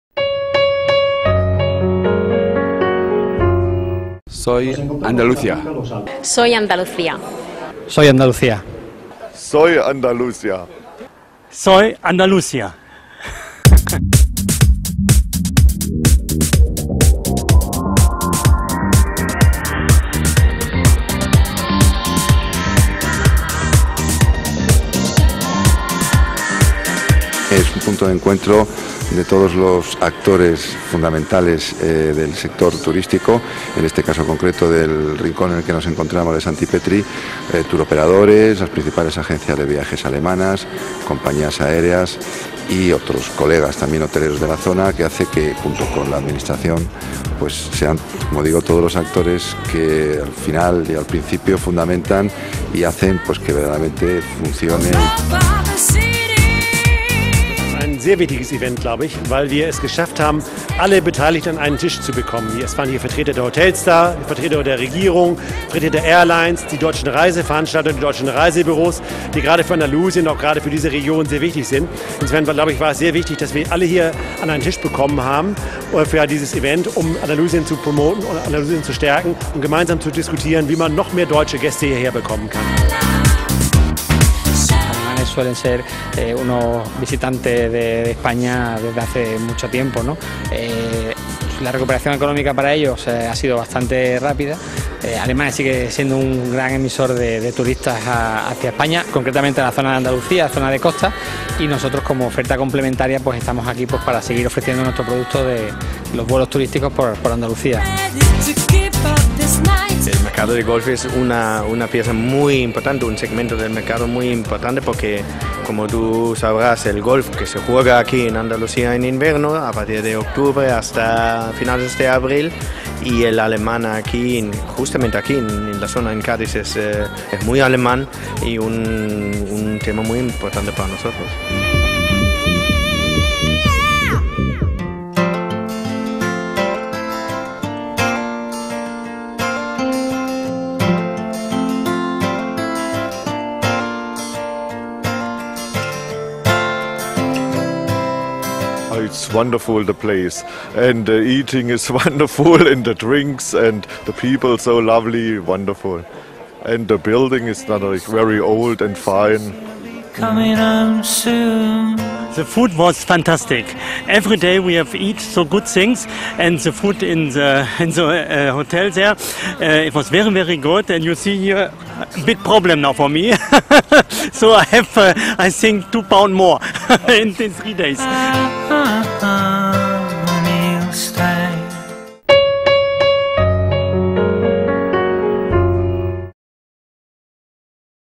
Alemanes en Jerez de la Frontera (Cádiz) (1 de 2)
Más de 50 turoperadores alemanes han aterrizado en Cádiz para conocer la diversidad turística que la provincia puede ofrecer. En este vídeo, podrán acompañarlos en los Workshops que se han llevado a cabo en Sancti Petri donde españoles y germanos del sector han compartido sus impresiones, y en la cena de gala en el majestuoso castillo de San Marcos en el Puerto de Santa María